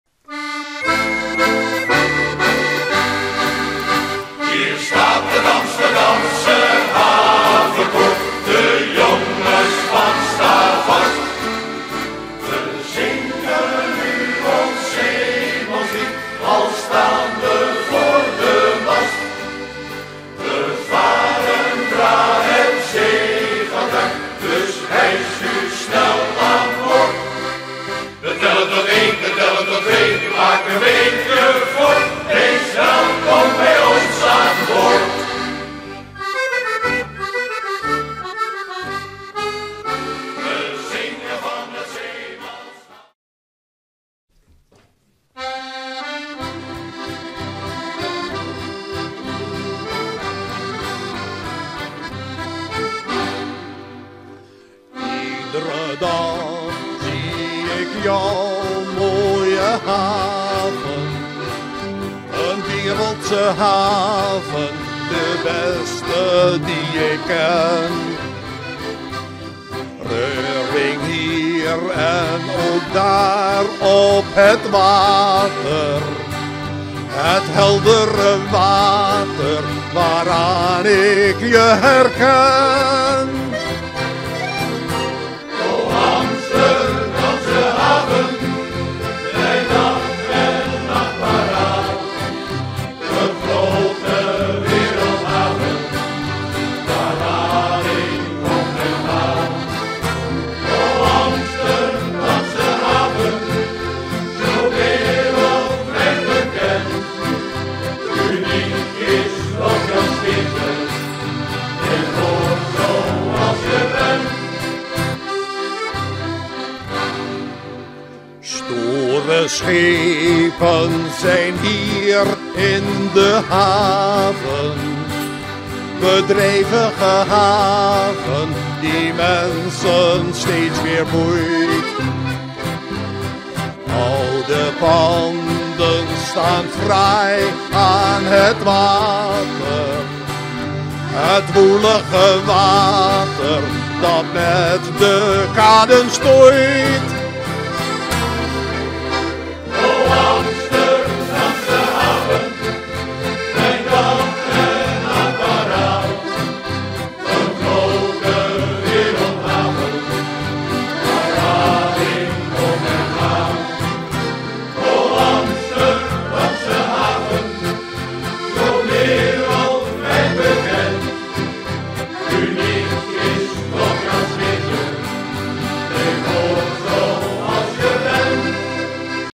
Havenlied 2010 Havenkoor.mp4
Het Amsterdams Havenkoor heeft meegedaan aan de wedstrijd voor een nieuw havenlied 2010.
U ziet in dit filmpje een korte impressie van de repetities van het koor.
Voor het volledige lied moet u zeker eens gaan kijken bij een van de regelmatige optredens.
Boeken van dit unieke koor kan natuurlijk ook. Voor informatie Kom ook kijken/luisteren
 naar dit geweldige koor.